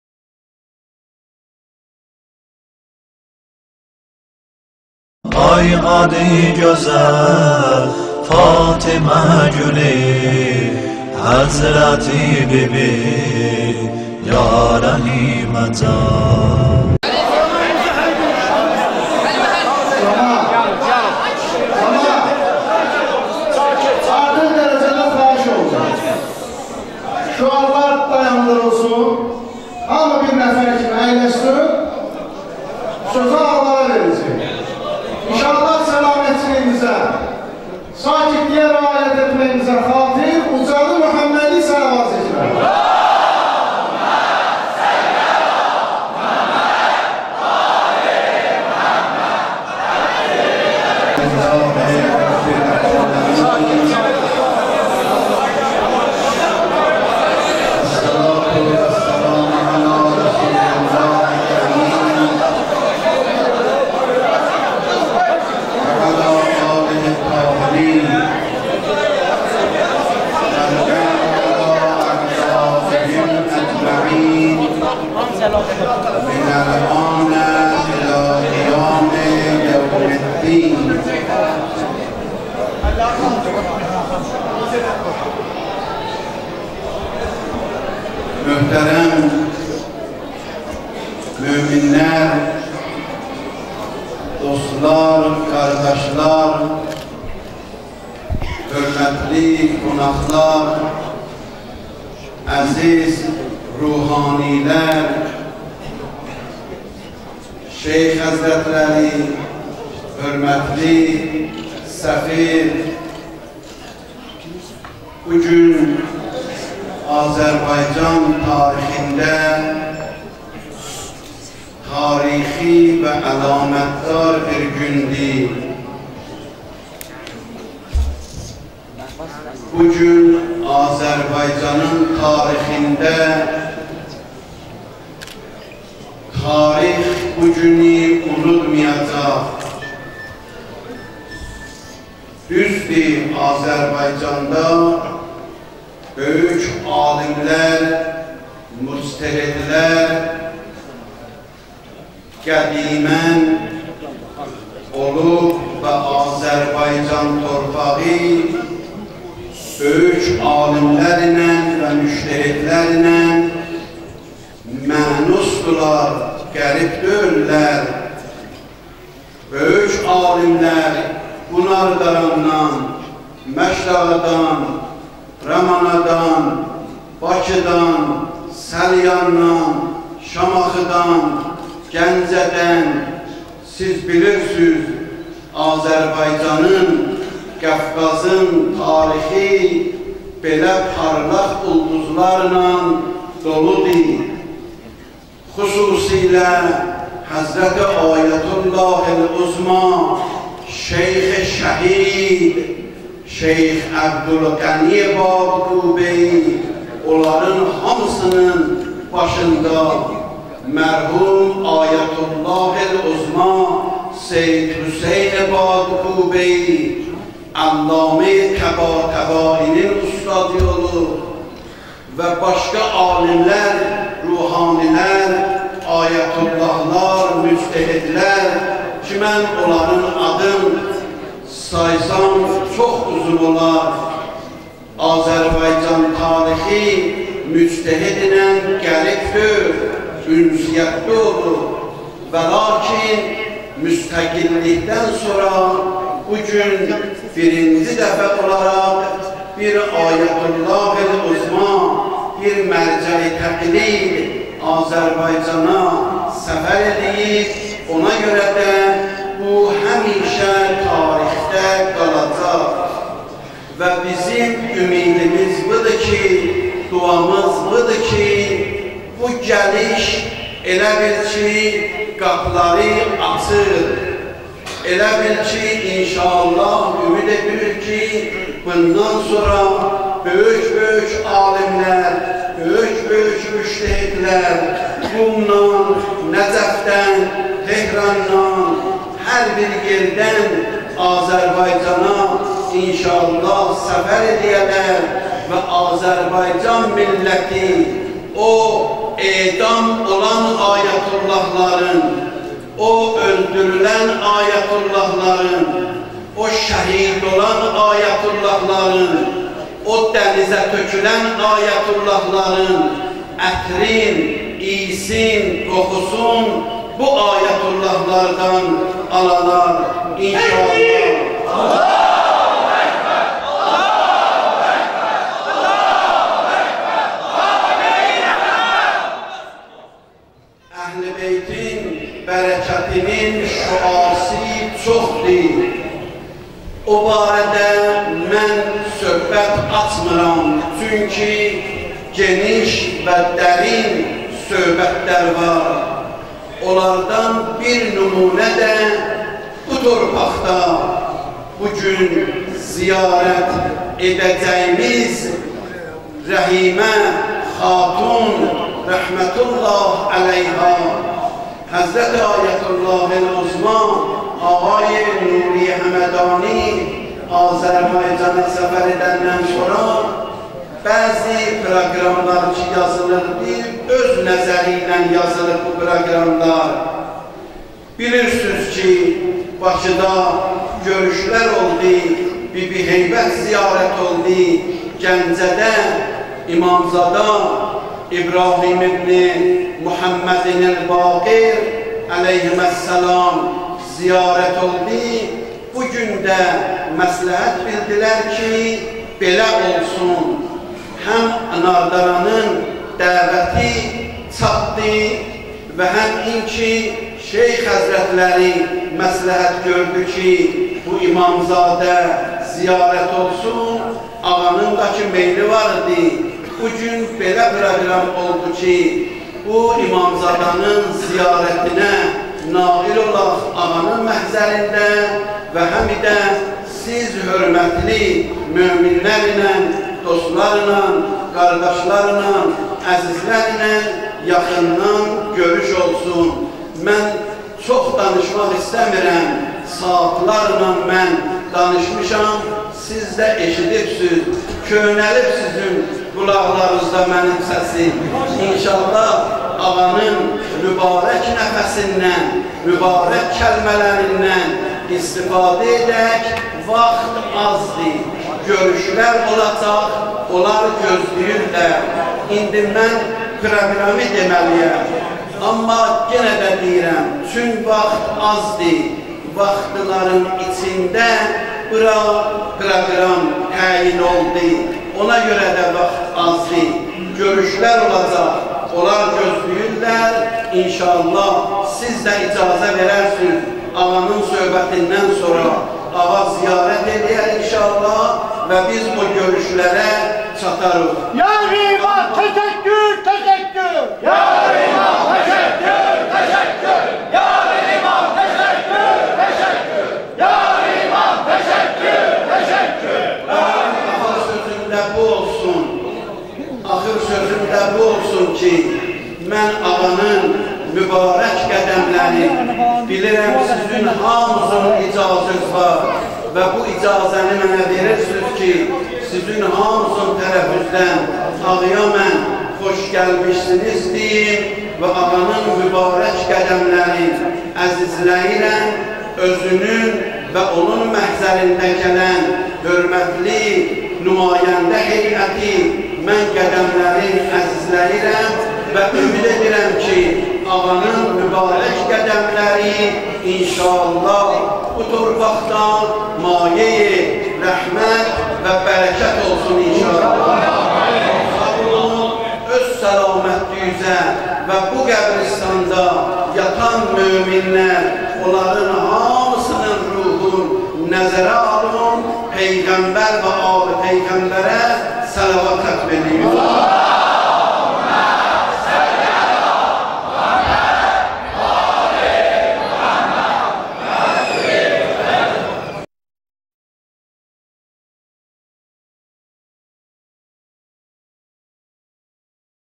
Nardaran Rəhimə xanım (s.ə) ziyarətgahında Hüccətul islam vəl muslimin Ocaq Nejat moizə edir.
Nardaranpiri.com: Nardaran Rəhimə xanım (s.ə) ziyarətgahının HüseyniyyəsindəAyətullah Nuri Həmədani cənablarını müşaiət edən  Hüccətul islam vəl muslimin Ocaq Nejat moizə edir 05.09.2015